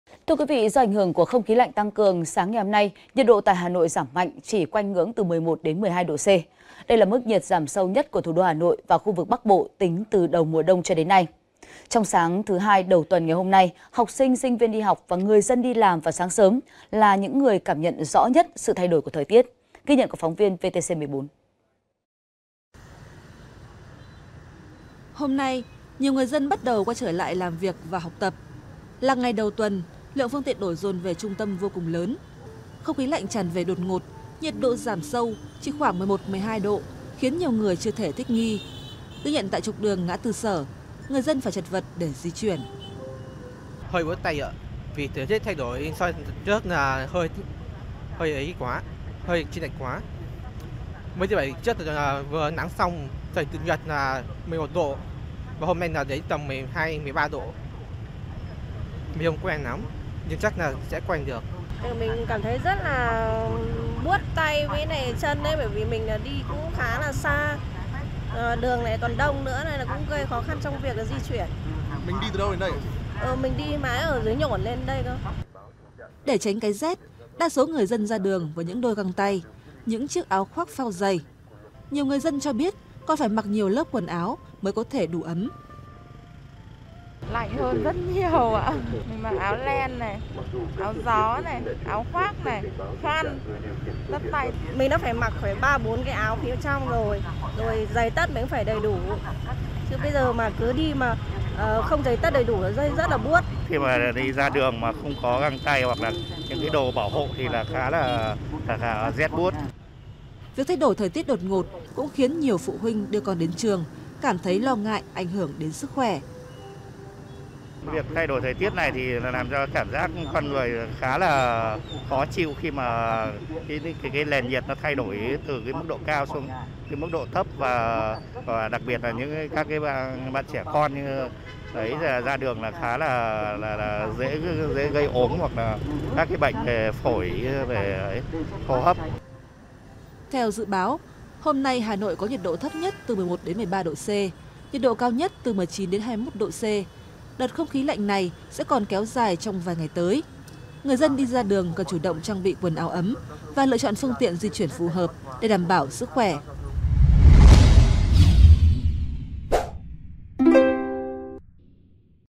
Người dân Hà Nội chật vật đi làm trong giá rét sáng đầu tuần |VTC14 GREEN
VTC14 GREEN| Do ảnh hưởng của không khí lạnh tăng cường, sáng nay, nhiệt độ tại Hà Nội giảm mạnh, chỉ quanh ngưỡng 11-12 độ C. Đây mà mức nhiệt giảm sâu nhất của thủ đô Hà Nội và khu vực Bắc Bộ tính từ đầu mùa đông đến nay. Trong sáng thứ đầu tuần hôm nay, HS-SV đi học và người dân đi làm  vào sáng sớm là những người cảm nhận rõ sự thay đổi của thời tiết.